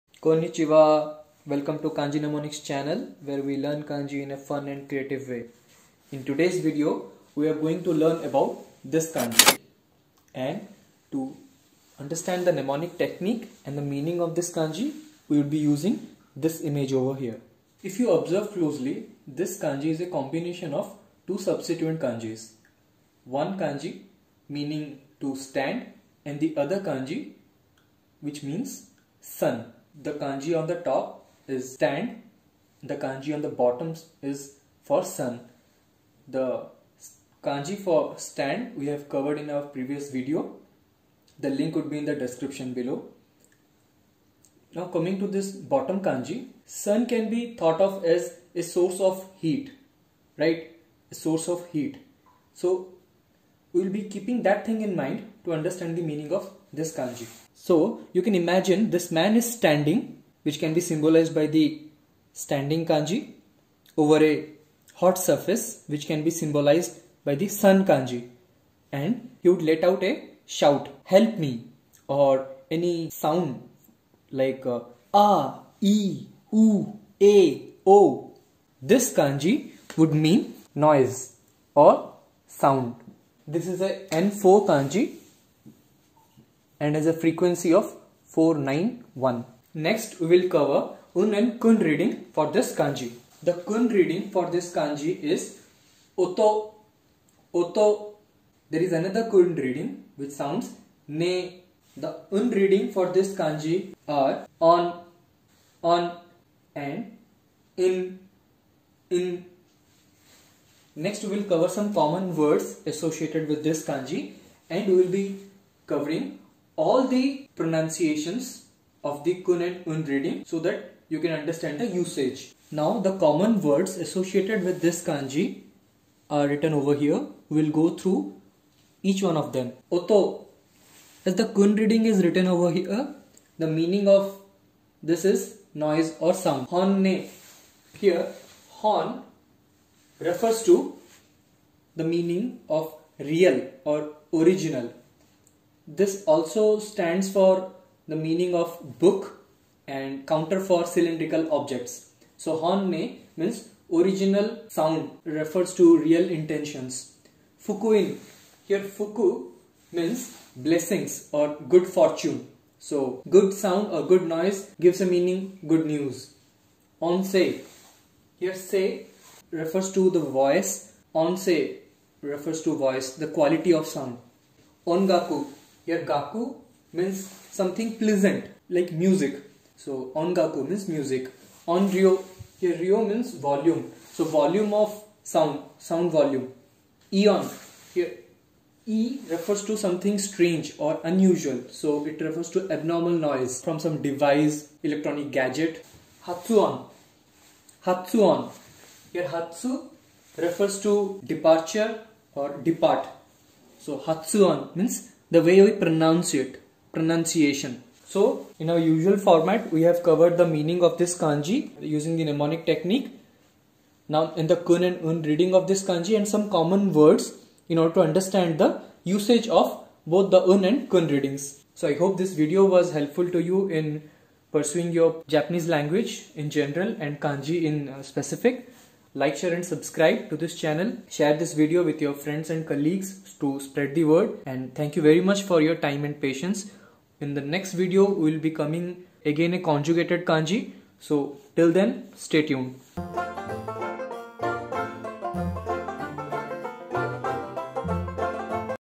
REMEMBER JAPANESE KANJI FAST AND EASY : 音 (NOISE, SOUND) || VLOG 17
After watching this video, you wouldn't find Kanji difficult, anymore!

We will use Mnemonic technique in this video to remember Kanji in a FUN and CREATIVE way.

音 (NOISE, SOUND) : JLPT N4, 491
~The number above refers to FREQUENCY of appearance of that Kanji in literature or otherwise.

In a series of videos we would cover as much as of them as possible so that you don't have to worry about Kanji in upcoming JLPT exam.

Even one word would mean a lot to us. ～☆